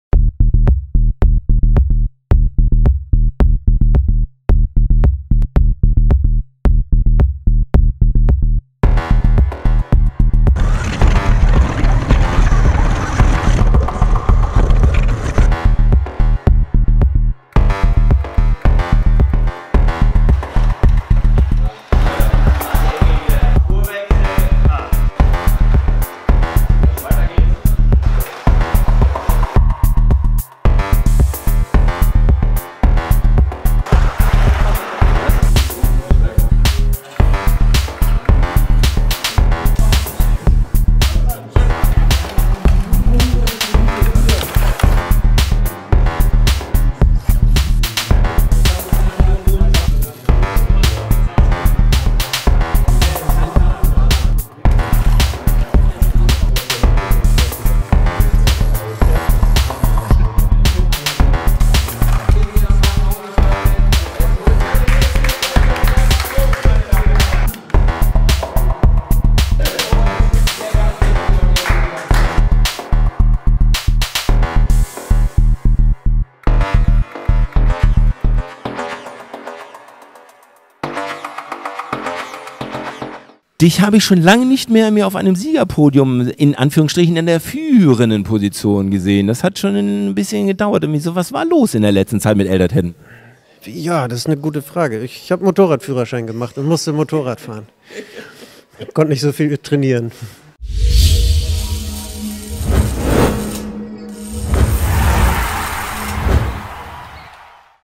RCCO 2019 | Lauf 3 | CityCup | Highlights
Zweiter Lamborghini-Sieg in Folge in der "großen kleinen" Rennserie: Von der Pole-Position aus sicherte sich Eldert Hedden (Joest Racing) den Sieg beim CityCup im Automuseum PROTOTYP. In einem packenden Finale setzte er sich mit 2:1 Siegen knapp gegen Titelverteidiger Thomas Voigt durch. Der Audi-Pilot übernahm mit Platz zwei die Tabellenführung. Das Podium komplettierte Björn Skottke (KTM).